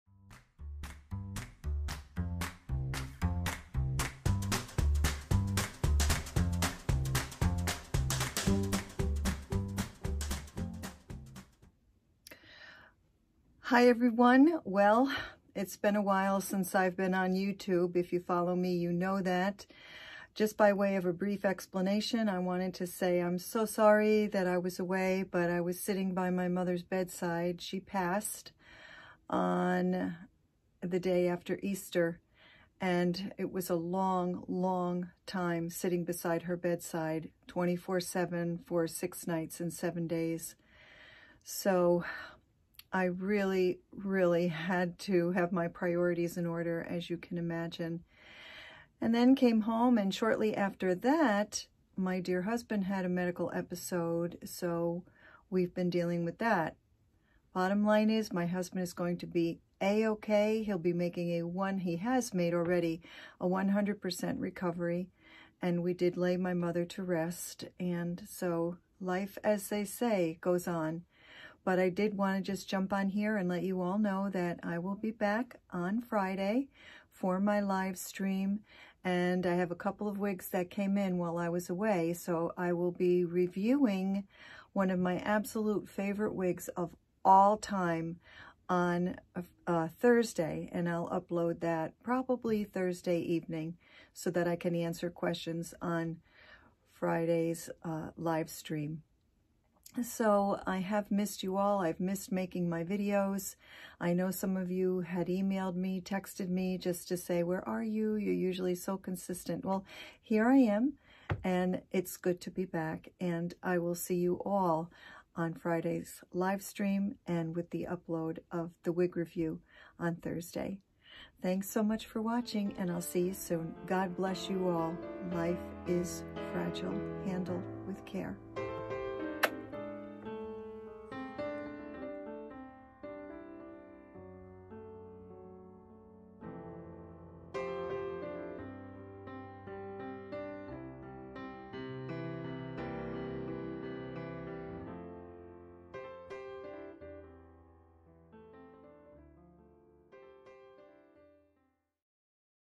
When Life hands you an unexpected challenging surprise! | New Live Stream This Friday @3:00 EST
When Life hands you an unexpected challenging surprise!  I'm Back After Being Away
Mother Has Passed and Other things.  Live Stream This Friday @3:00 EST